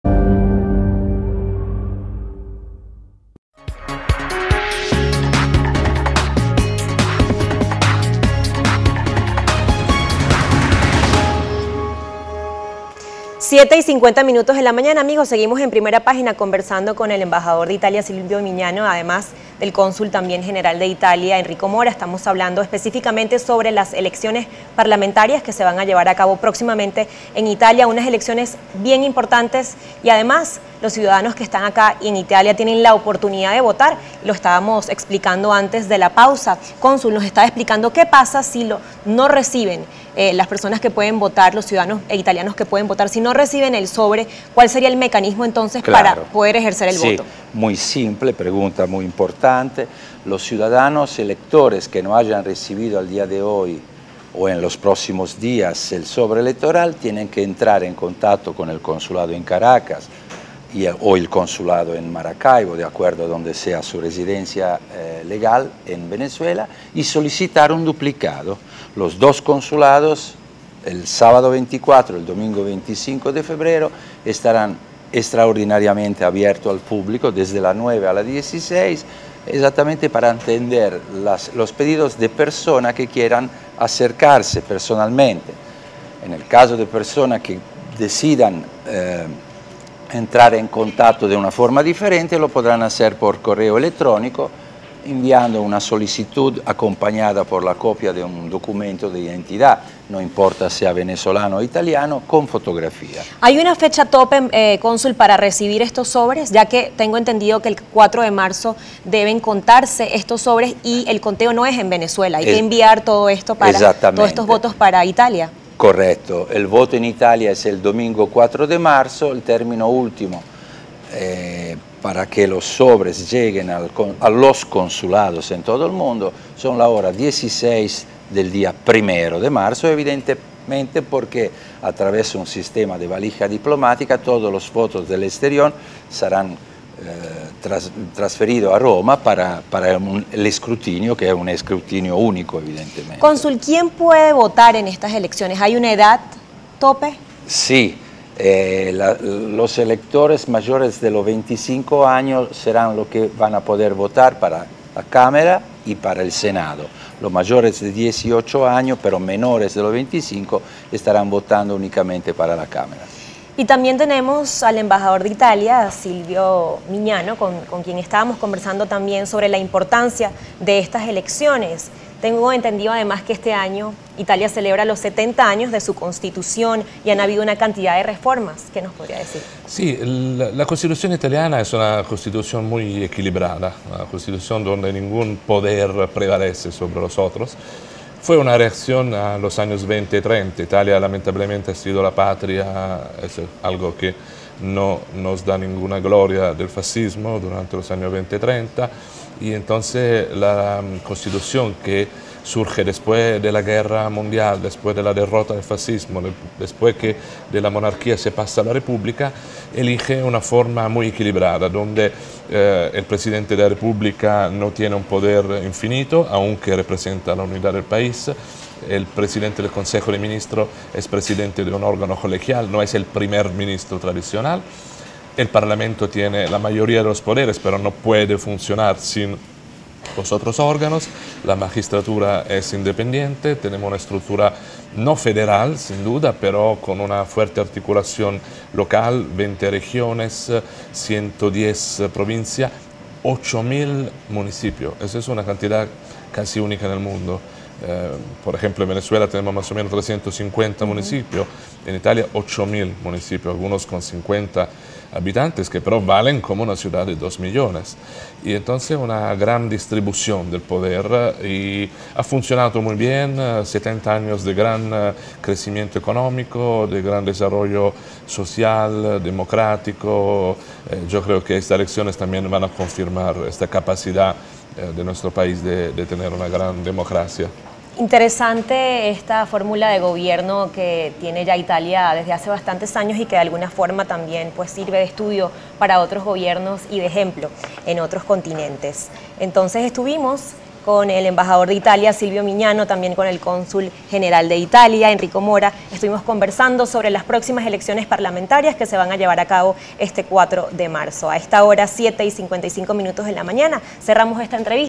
Silvio Mignano: Desde 2001 italianos en el exterior del país pueden votar (Parte 2/2)
El embajador de Italia Silvio Mignano informó que en las elecciones parlamentarias que se llevarán a cabo este próximo 14 de marzo en Italia, son unas elecciones en donde los italianos residenciados en Venezuela ejercer su derecho al voto